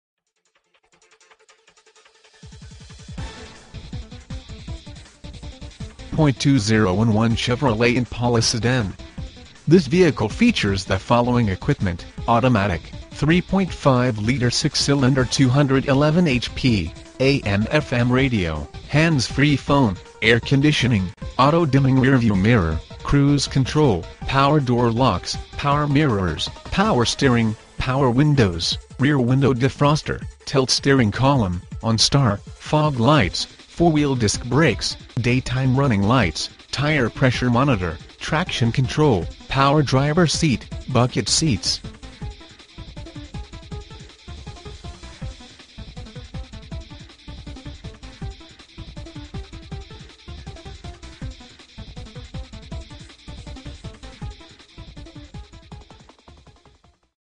2011 Chevrolet Impala - Cal's @ 5th ave - Anchorage, AK 99501
Cal's @ 5th ave
431 unga street

For Sale: 2011 Chevrolet Impala
VIN: 2G1WG5EK3B1305134
Engine: 3.5-Liter 6-Cylinder 211HP
Drivetrain:
Transmission: automatic
Mileage: 26,827
Color: SILVER (exterior)  (interior)